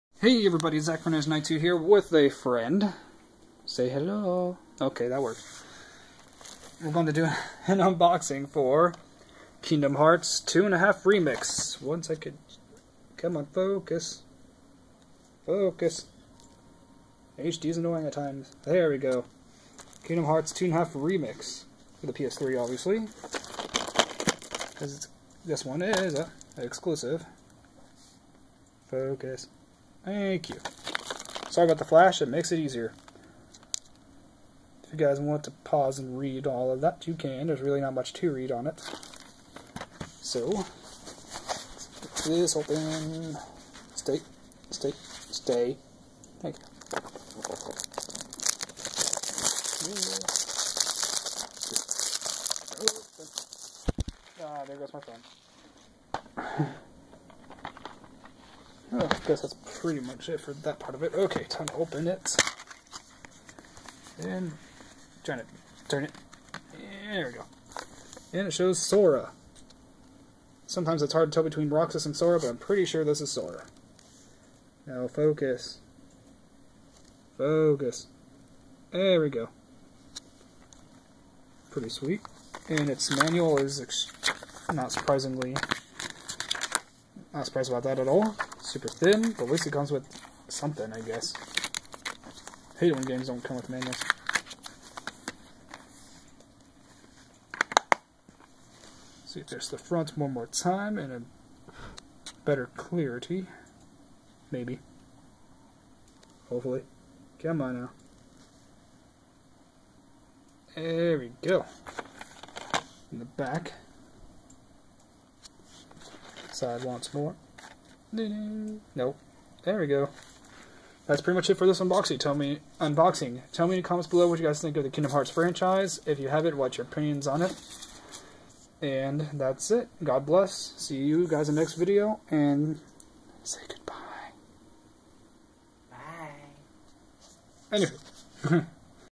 Kingdom Hearts: 2.5 Remix - PS3 Unboxing!
Kingdom Hearts: 2.5 Remix - PS3 Unboxing! Enjoy =) HD is awesome. I liked this series, but never beat a single one. But now I have a chance to beat it. God Bless =)